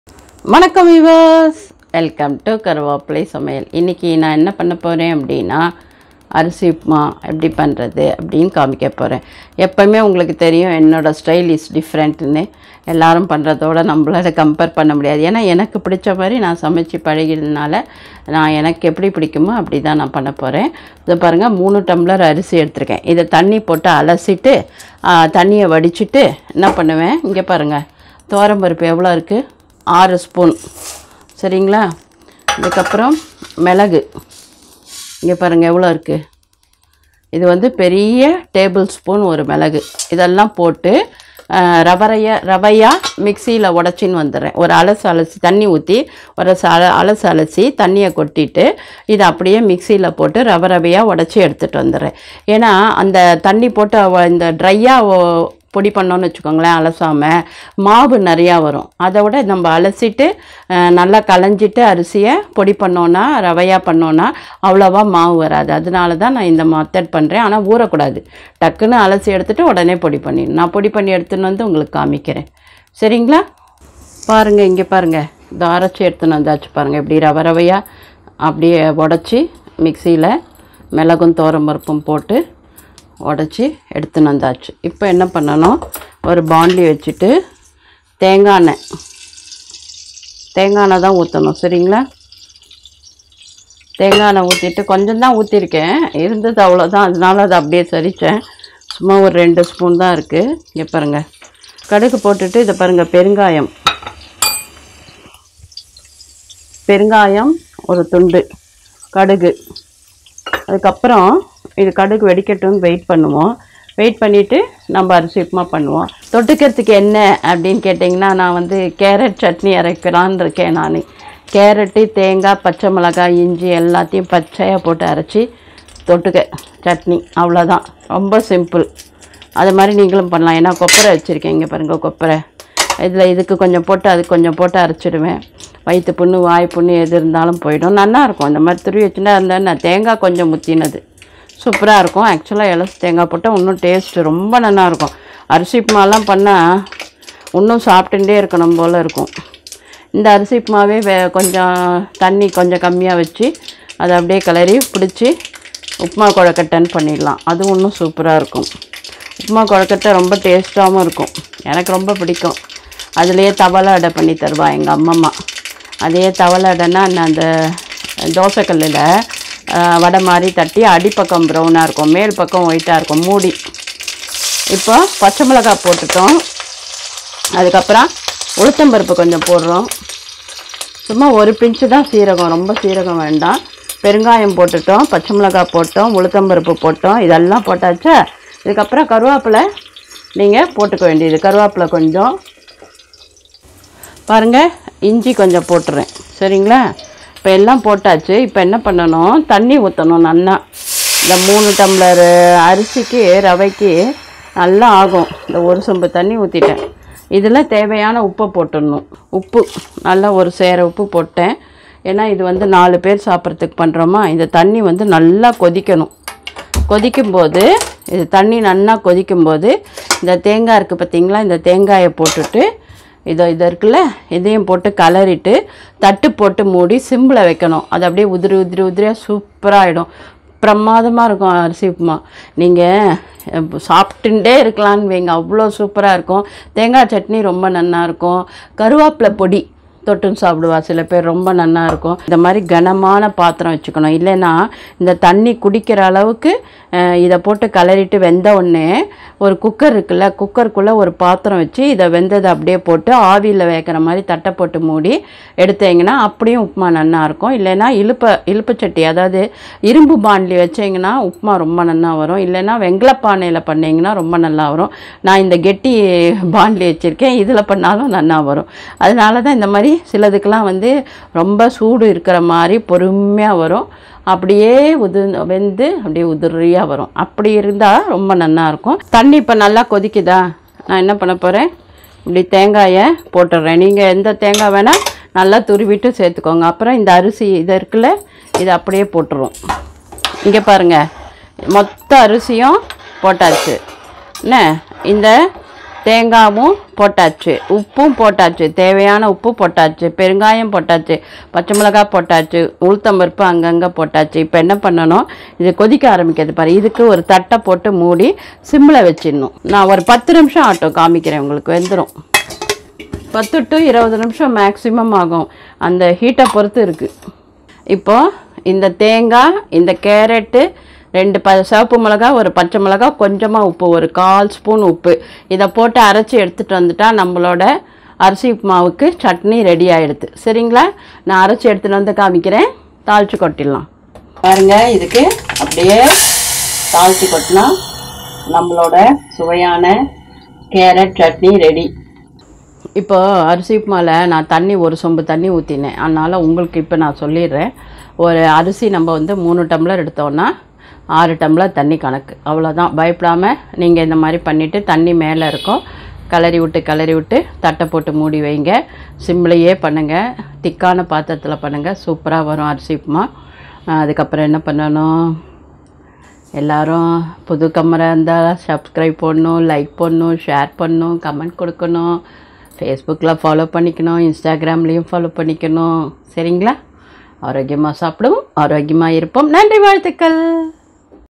சுவையான அரிசி உப்புமாவும் உடனடி சட்னியும் | Arisi Upma and Quick Chutney
இன்றைய பதிவில் நாம் சுவையாக அரிசி உப்புமா எப்படி செய்வது என்று பார்ப்போம் உப்புமா மட்டும் போதுமா மா னு நீங்க கேட்கிறது எனக்கு புரியுது அதனால தான் ஒரு சட்னியும் செய்திட்டேன்.

சுவையான அரிசி உப்புமாவும் உடனடி சட்னியும் | Arisi Upma and Quick Chutney @KaruveppilaiSamayal

(Please Note: Outside Chennai Delivery Charges have to paid seperately)